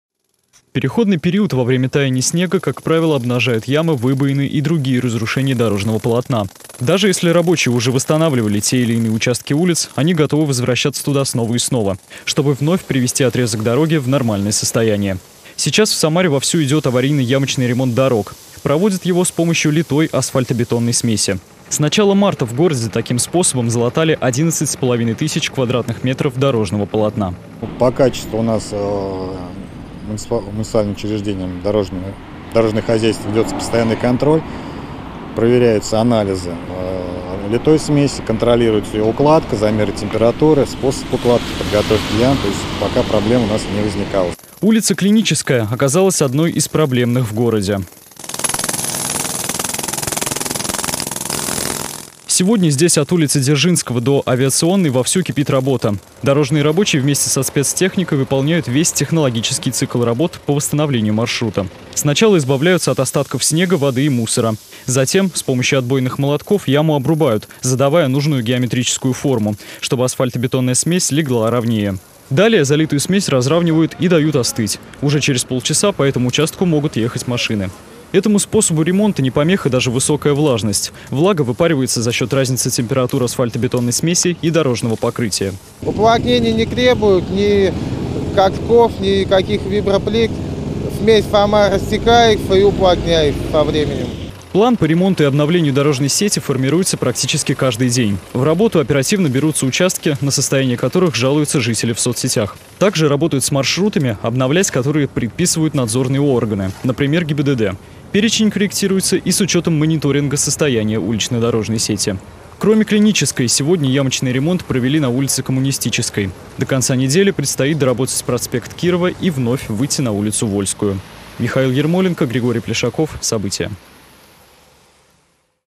В Самаре вовсю идет аварийно-ямочный ремонт дорог
В Самаре отремонтировали дороги на улице Клинической и Коммунистической, в планах - проспект Кирова и улица Вольская. Ямы и выбоины устраняют с помощью литой асфальто-бетонной смеси. С предприятиями, которые занимаются содержанием дорог местного значения, заключили контракты уже и на второй квартал этого года. С технологией аварийно-ямочного ремонта ознакомился  Михаил Ермоленко.